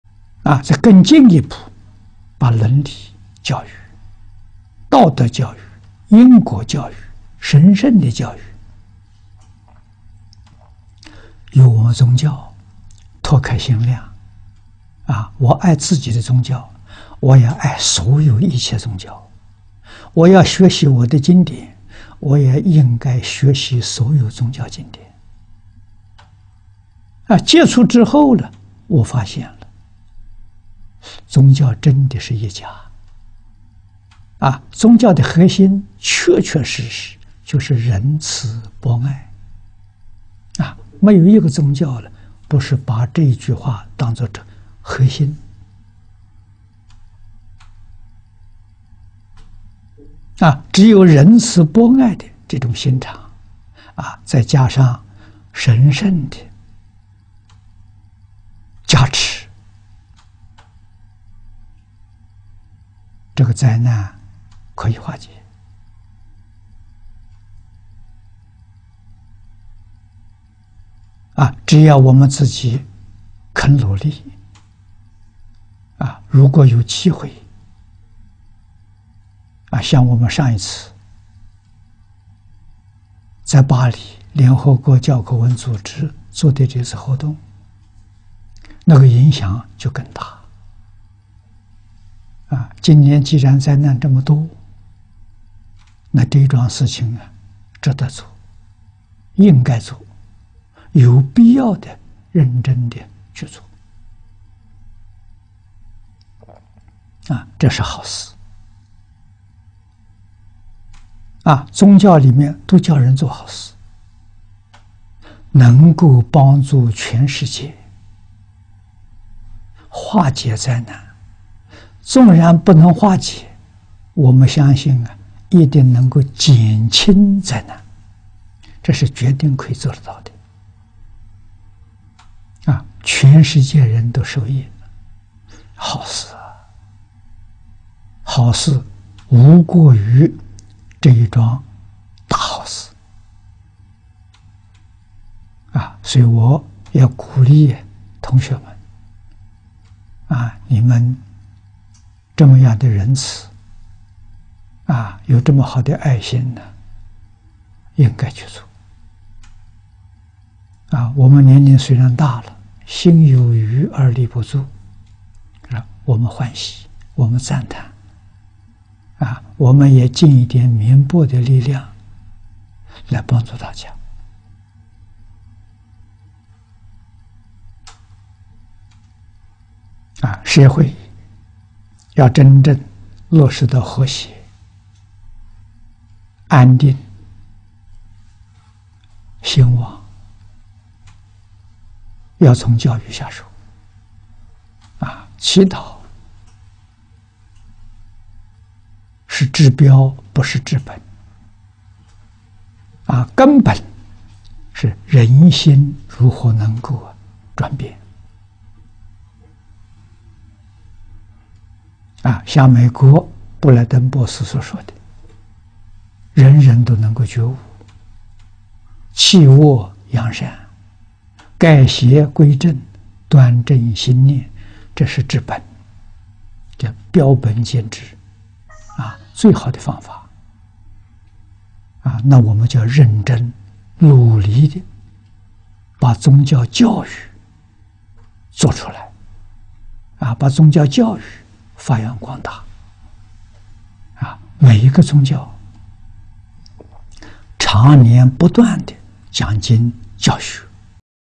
淨空老法師:「世界宗教是一家」開示 2/4
影片字幕：
把倫理教育、道德教育、因果教育、神聖的教育，由我們宗教拓開心量，我愛自己的宗教，我也愛所有一切宗教；我要學習我的經典，我也應該學習所有宗教經典。接觸之後我發現了，宗教真的是一家，宗教的核心確確實實就是仁慈、博愛，沒有一個宗教不是把這句話當作核心。只有仁慈博愛的這種心腸，再加上神聖的加持，這個災難可以化解，只要我們自己肯努力。

       如果有機會，像我們上一次在巴黎聯合國教科文組織做的這次活動，那個影響就更大。今年既然災難這麼多，那這樁事情值得做、應該做，有必要認真的去做，這是好事。宗教裡面都教人做好事，能夠幫助全世界化解災難，縱然不能化解，我們相信一定能夠減輕災難，這是決定可以做得到的。全世界人都受益，好事！好事無過於這一樁大好事。所以我也鼓勵同學們，你們這麼樣的仁慈，有這麼好的愛心，應該去做。我們年齡雖然大了，心有餘而力不足，但是我們歡喜、我們讚歎，我們也盡一點綿薄的力量來幫助大家。

       社會要真正落實到和諧、安定、興旺，要從教育下手。祈禱是治標，不是治本，根本是人心如何能夠轉變。像美國布萊登博士所說的，人人都能夠棄惡揚善、改邪歸正、端正心念，這是治本。標本兼治，最好的方法。那我們就要認真努力的把宗教教育做出來，把宗教教育發揚光大，每一個宗教長年不斷的講經教學。